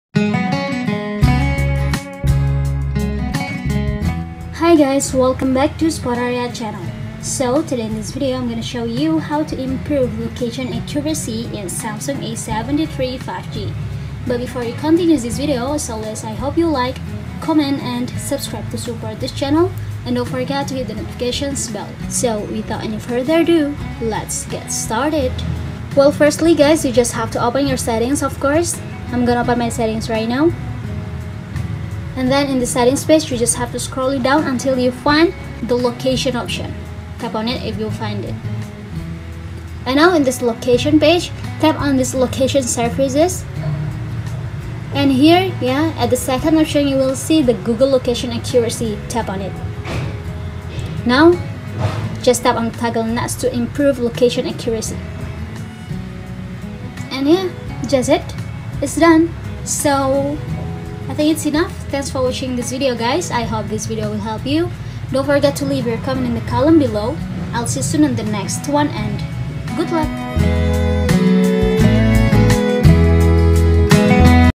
How To Improve Location Accuracy Samsung A73 5G
How to improve location accuracy samsung a73 5g - The level of accuracy of location on Samsung may vary from one phone to another. But at least if you want to improve location accuracy, you can activate the improve location accuracy feature via settings on Samsung.

Then how to do it? You can follow the steps to activate or increase accuracy on Samsung as I have shown in this video.